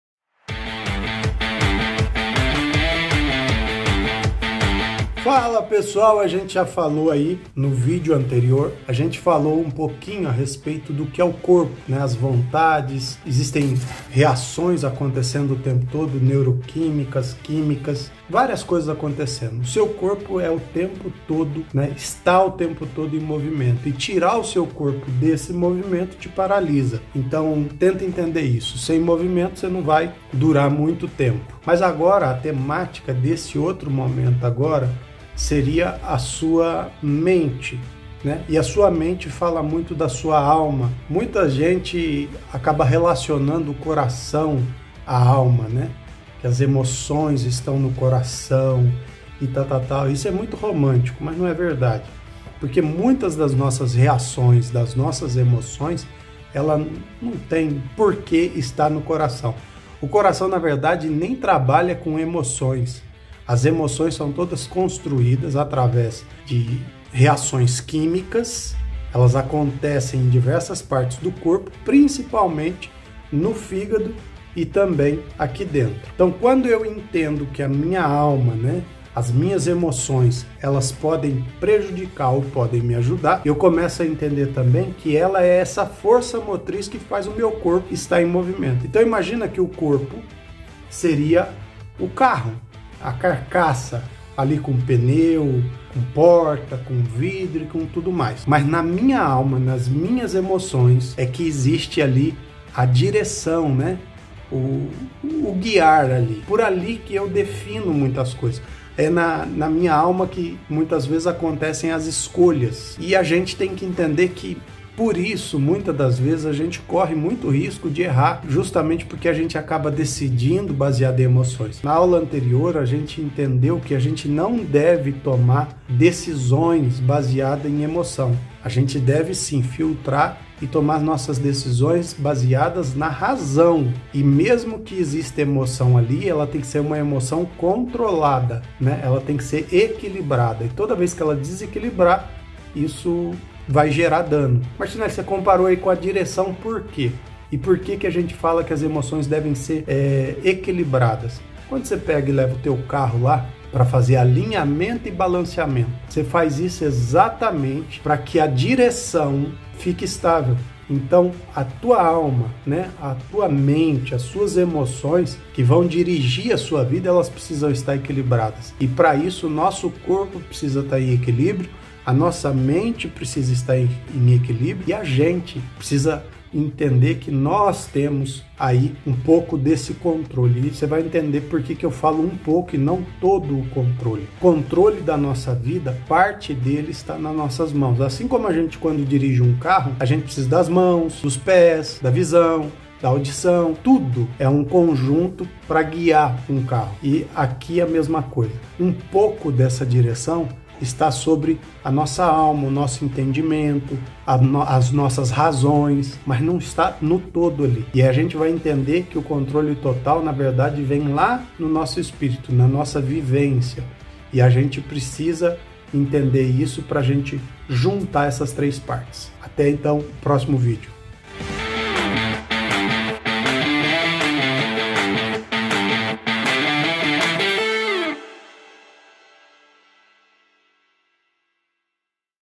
AULA 4.1 SALA7 - CORPO - ALMA - ESPIRITO
Agora vamos falar um pouco sobre essa segunda parte de quem somos, vamos entender o que é a alma e onde se concentram as emoções.